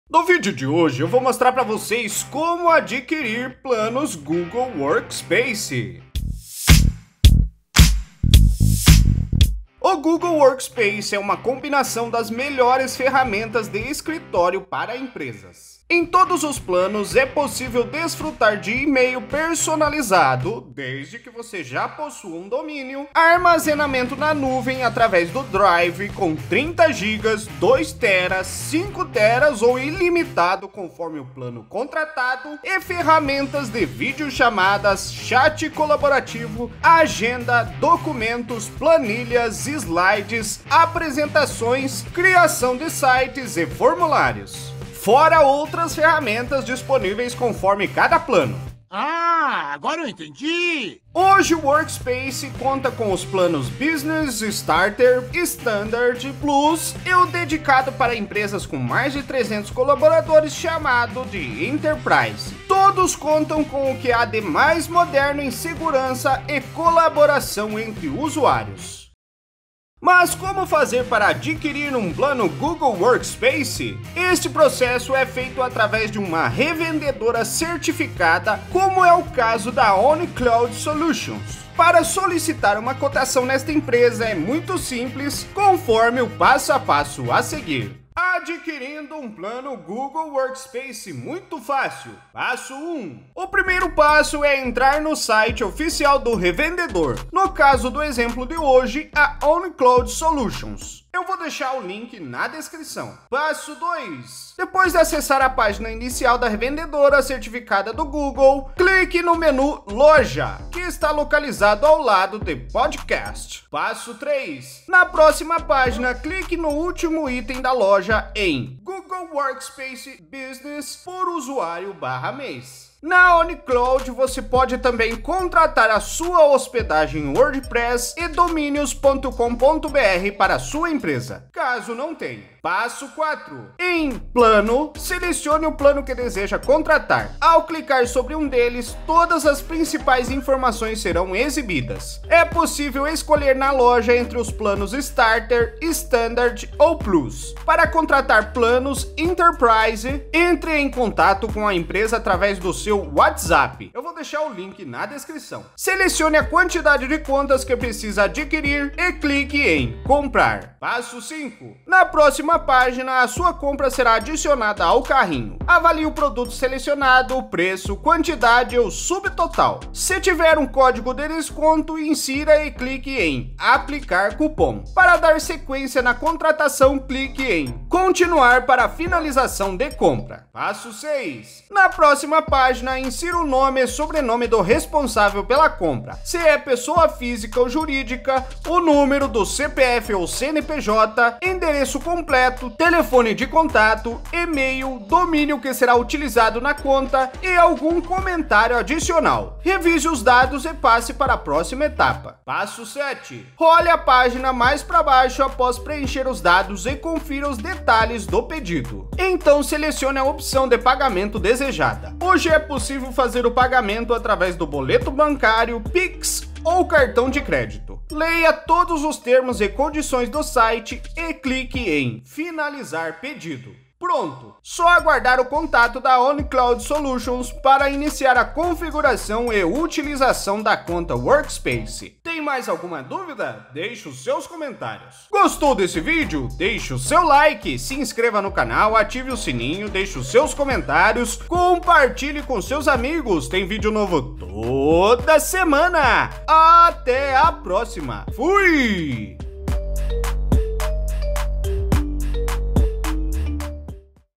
Como comprar e adquirir planos Google Workspace
Quer adquirir um plano Google Workspace para sua empresa ou para fins pessoais? Aprenda passo a passo como comprar planos Starter, Standard, Plus e Enterprise para pequenas, médias e grandes corporações na OneCloud Solutions.

Os planos Workspace são ferramentas de escritório para aumentar a produtividade e colaboração entre os funcionários. É também a melhor forma de profissionalizar a experiência de usuário e clientes, com email profissional, videoconferências, chat sem precisar sair do email, documentos, planilhas, formulários, agenda, criação de sites, armazenamento na nuvem e muito mais. Tudo isso em um único local.

Aprenda no @Comofazerfacilonline

♥️ Deixe o seu "Valeu" no vídeo.

Como fazer?

Capítulos:

00:00 - Abertura
00:06 - Intro
00:10 - Assunto
00:51 - Planos Workspace
01:10 - O que é preciso para adquirir um plano Google Workspace?
01:28 - Adquirindo um plano Workspace muito fácil
01:32 - Passo um
01:43 - Passo dois
01:53 - Passo três
02:12 - Passo quatro
02:40 - Passo cinco
03:00 - Passo seis
03:22 - Passo sete
03:55 - Final